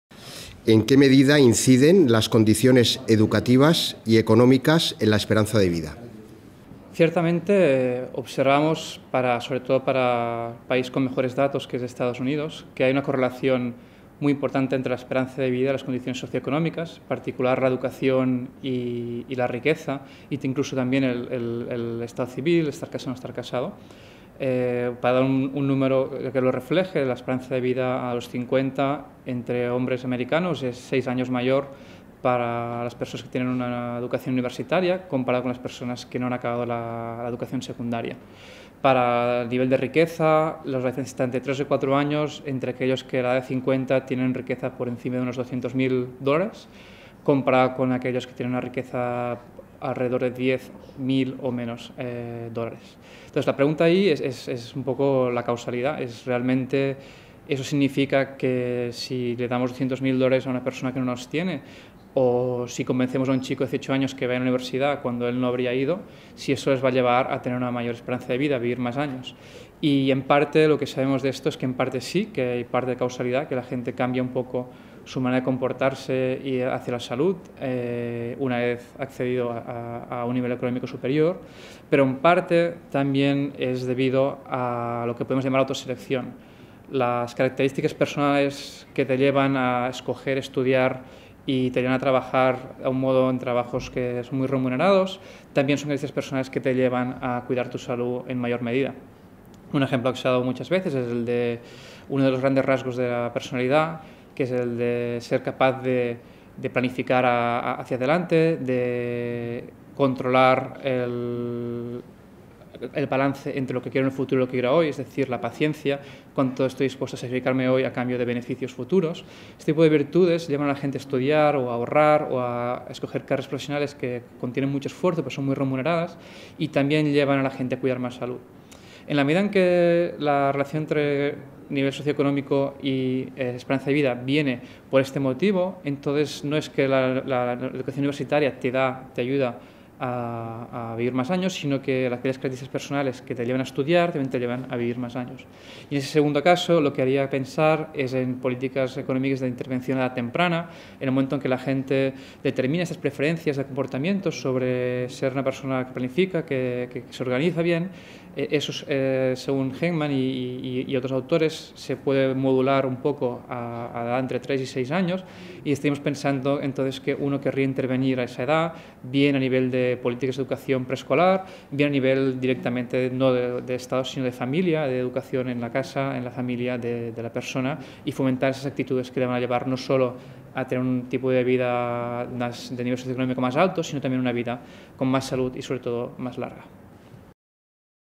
Globalización y equidad en salud
Josep Pijoan-Mas, del Centro de Estudios Monetarios y Financieros (CEMFI) de Madrid, nos habla de la influencia del determinante socio-económico sobre la salud: globalización y equidad.